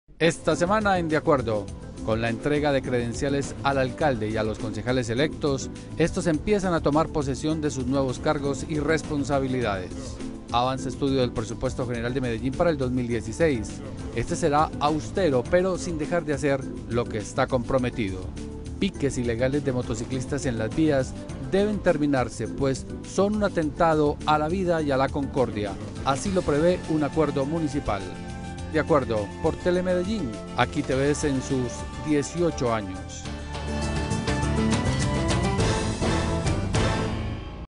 Avances De Acuerdo, noviembre 11
Con la entrega de credenciales al Alcalde y Concejales electos, estos empiezan a tomar posesión de sus nuevos cargos y responsabilidades. Avanza estudio del Presupuesto general de Medellín para 2016. Éste será austero pero sin dejar de hacer lo que está comprometido.Piques ilegales de motociclistas en las vías deben terminarse pues son un atentado a la vida y la concordia; así lo prevé un Acuerdo Municipal.